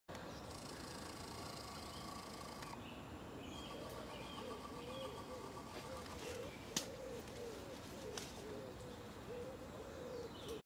Fallow deer
A few fallow deer in a bunch of brambles